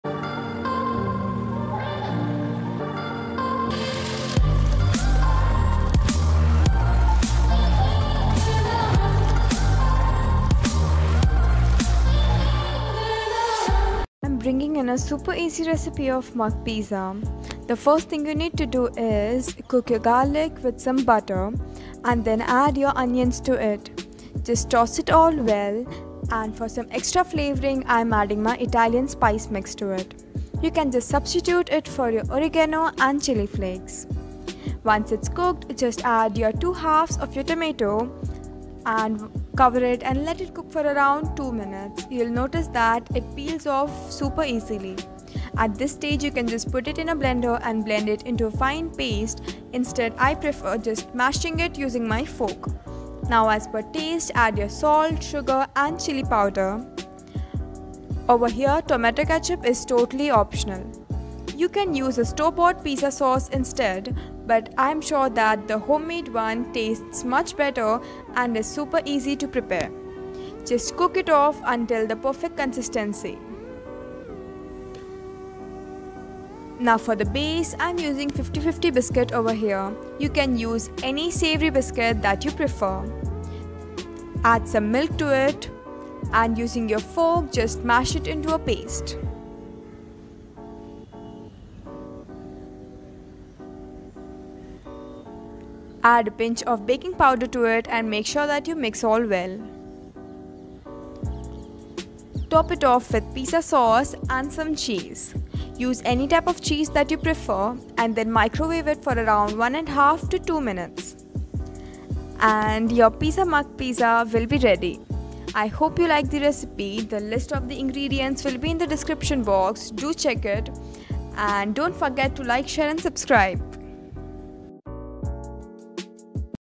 Mug pizza using just 3 ingredients - super easy and delicious snack| Blend
Mug recipes were always my favourite because it's so easy and quick yet so delicious and satisfying at the same time. This is a one serve recipe.Just put together these three thing - cookies(base), sauce , and cheese and voila. You just need 5 minutes to create this recipe, and your super yummy pizza will be ready. I know how much everyone is missing their pizzas(including me). I hope this helps you to satisfy your cravings.

INGREDIENTS

10-12 savoury cookies
3 tbsp milk
A pinch of baking powder

FOR THE PIZZA SAUCE
4-5 garlic
1/4 onion
1 medium sized tomato
Oregano
Chilli flakes/chilli powder
1/2tbsp Sugar (as per taste)
1/2tbsp Salt (as per taste)

FOR TOPPINGS
Go as creative as you can with toppings( there are no limits)
Cheese (as much as you wish)

If you recreate it, dont forget to tag me.

Try it out n let me know what you think about it in the comments section.